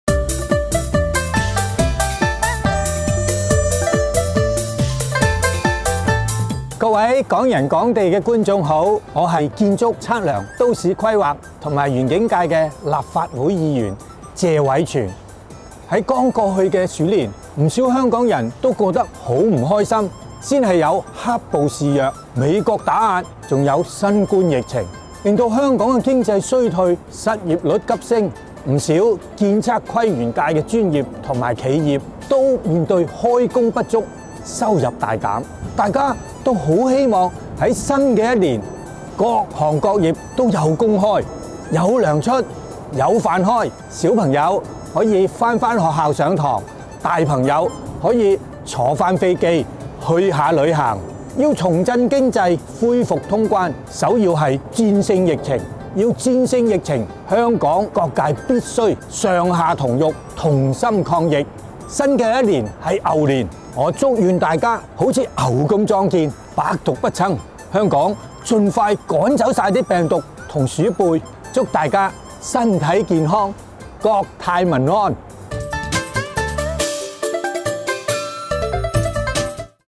【短片】【牛咁健壯、百毒不侵】美國打壓、疫情​夾擊經濟 謝偉銓：戰勝疫情要香港各界上下同欲、同心抗疫！
//希望香港好似牛咁樣強強壯，Fight低所有困難！//